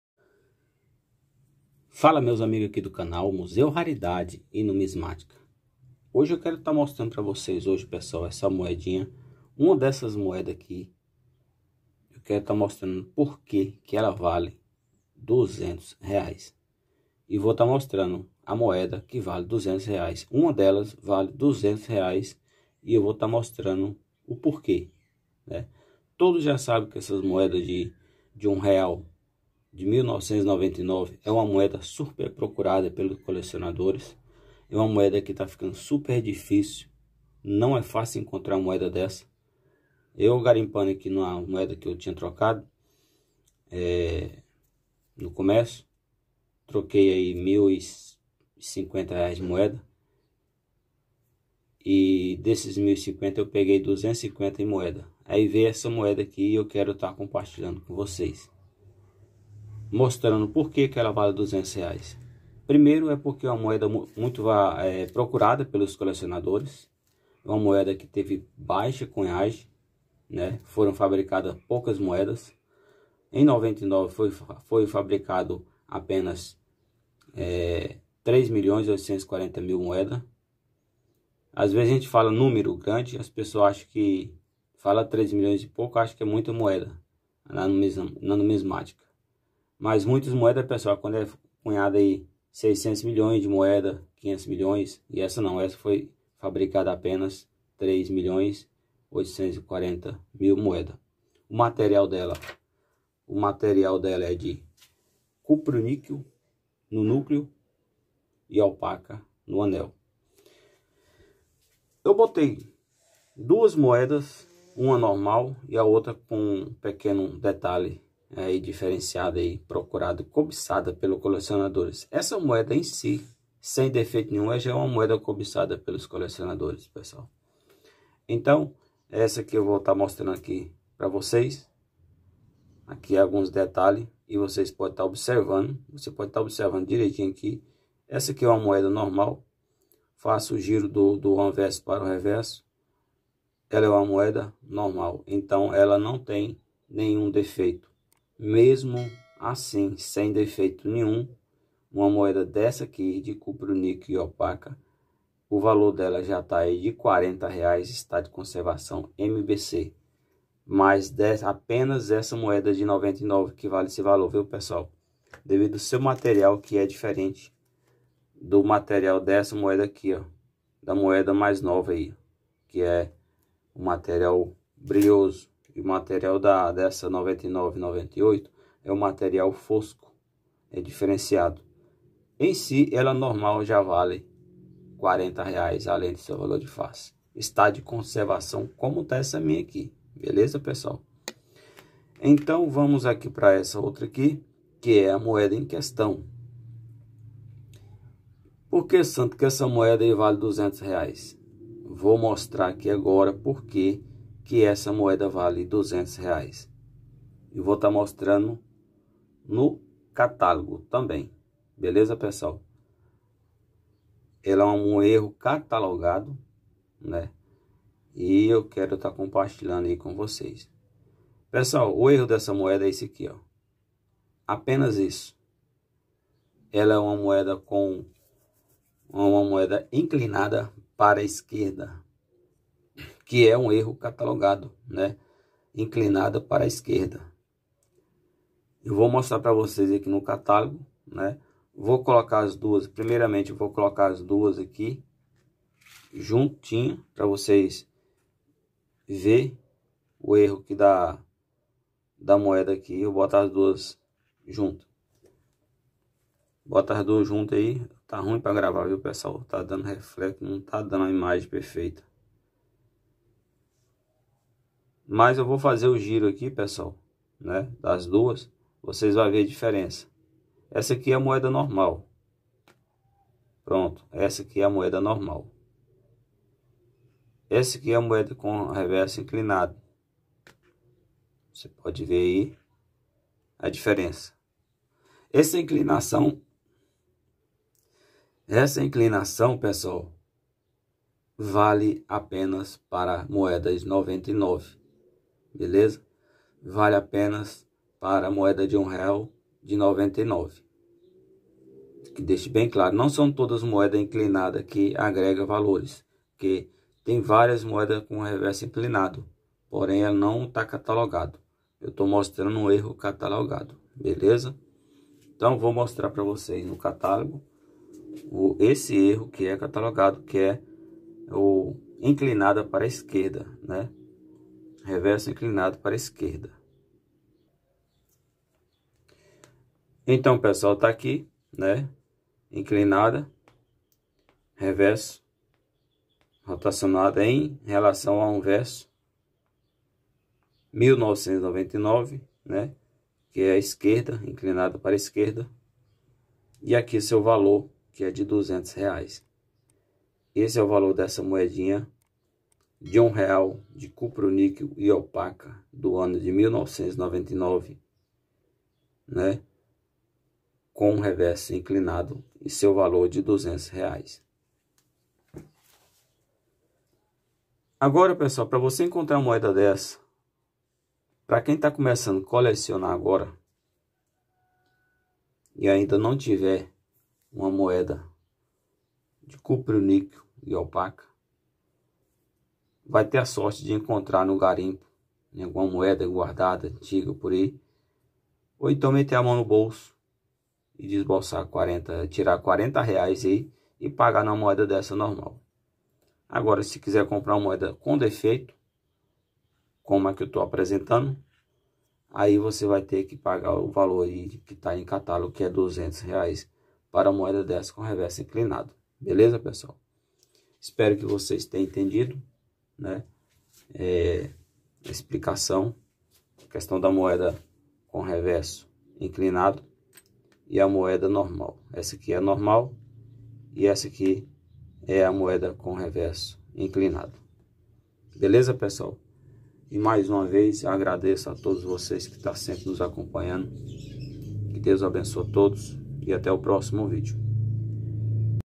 200 REAIS MOEDA VALIOSA DE 1 REAL 1999 CUPRO-NIQUEL E ALPACA🤑
SE QUISER UMA DESSA VAI TER QUE METER A MÃO NO BOLSO.

Aprenda avaliar suas moedas você pode ganhar uma boa grana

200 REAIS MOEDA VALIOSA DE 1 REAL 1999 CUPRO-NIQUEL E ALPACA🤑

Não troque suas moedas sem antes ver este vídeo aprenda a indentificar suas moedas se elas agregam  bons valores numismáticos MUSEU RARIDADE E NUMISMÁTICA SEMPRE PASSANDO I FORMAÇÕES ATUALIZADAS ATÉ VOCÊ